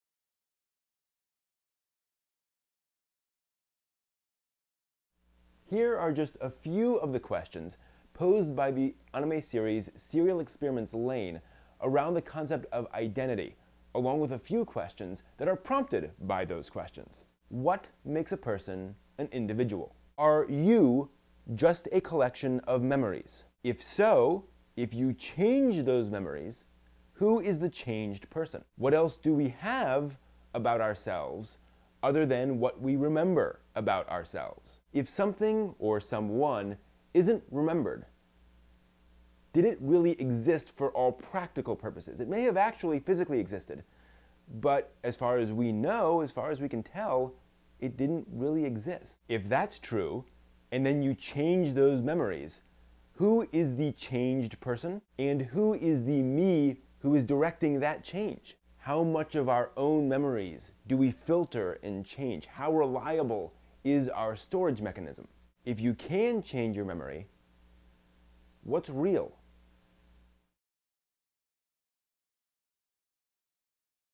serial experiments lain: Questions of Identity
Here are a few of the questions raised in the anime series "serial experiments lain" about the problem of identity and memory